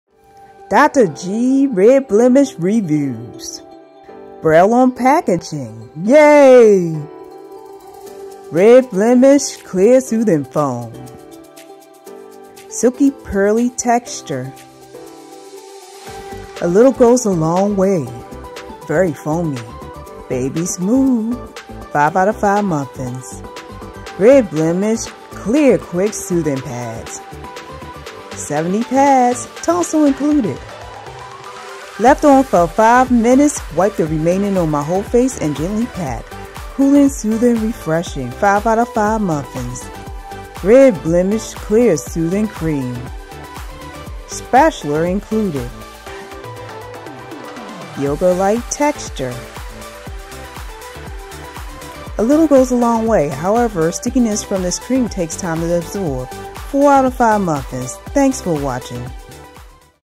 Dr. G R.E.D BLEMISH Reviews [Shorts with Voiceover]
Three products from Dr. G R.E.D BLEMISH line that I won through Picky to give my short honest reviews. 💜